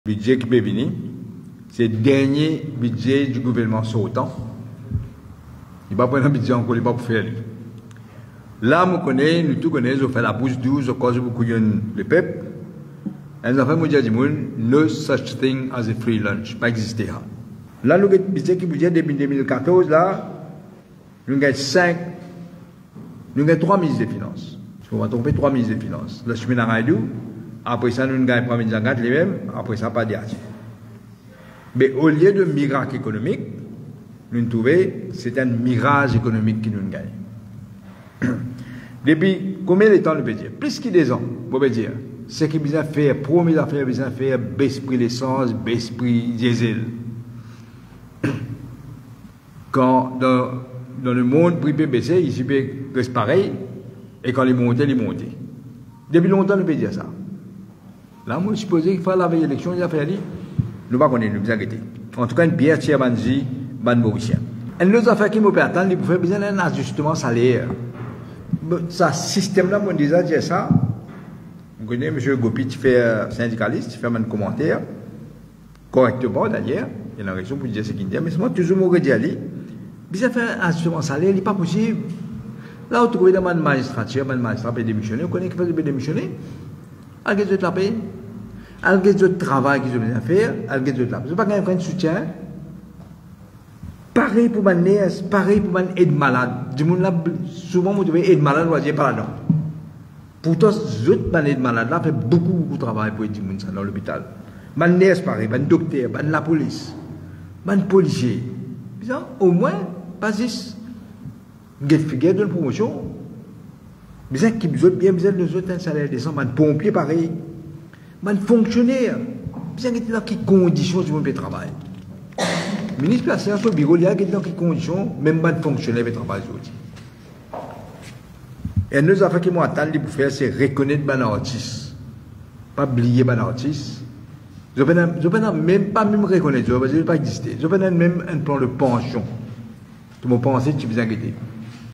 Sur les budgets du gouvernement MSM depuis 2014 : « Au lieu de miracle économique, c’est un mirage..
Sur les budgets du gouvernement MSM depuis 2014 : « Au lieu de miracle économique, c’est un mirage économique que nous avons eu jusqu’ici », affirme Navin Ramgoolam
Lors de son point de presse tenu hier après la réunion du bureau politique du Parti travailliste, Navin Ramgoolam s'est attardé sur la présentation du Budget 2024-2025 prévue pour ce vendredi 7 juin.
Pour le leader du Parti travailliste, le gouvernement n’aura d’autre choix que de faire des compromis avant d’ajouter que « There’s no such thing as a free lunch ».
Selon lui, depuis que le gouvernement MSM est au pouvoir, le pays n’aurait connu aucun miracle économique mais aurait plutôt dû faire face à un mirage économique. Navin Ramgoolam dit s’attendre à une baisse des prix de l’essence et du diesel.
Navin Ramgoolam espère également un réajustement des salaires pour les travailleurs. Il a aussi eu une pensée spéciale pour les artistes, soulignant qu'ils n’ont jusqu'ici reçu ni reconnaissance ni plan de pension.